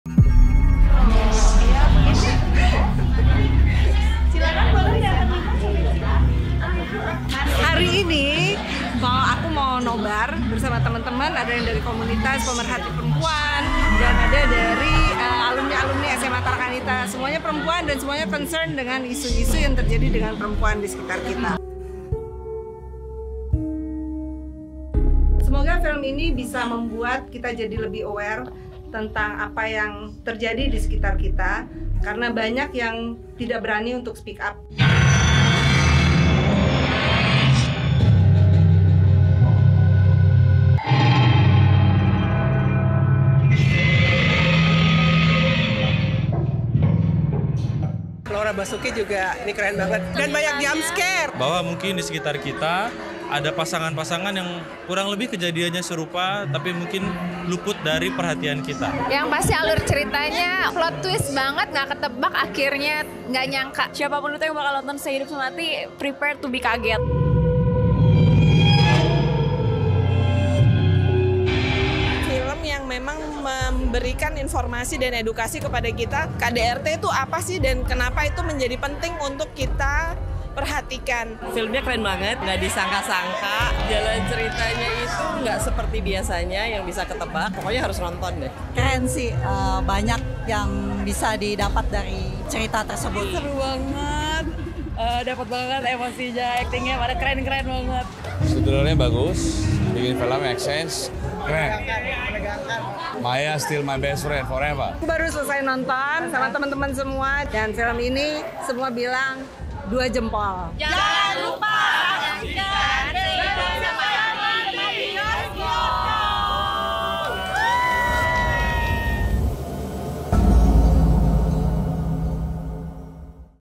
Nobar SEHIDUP SEMATI bersama komunitas Mindfulness Movement
SAKSIKAN DI BIOSKOP

Produser : Chand Parwez Servia, Fiaz Servia
Sutradara : Upi

Pemain:
Laura Basuki, Ario Bayu, Asmara Abigail, Chantiq Schagerl, Maya Hasan, Lukman Sardi, Whani Darmawan, Aqeela Dhiya, Ivanka Suwandi, Elly D Lutan, Verdi Solaiman, Patty Angelica Sandya.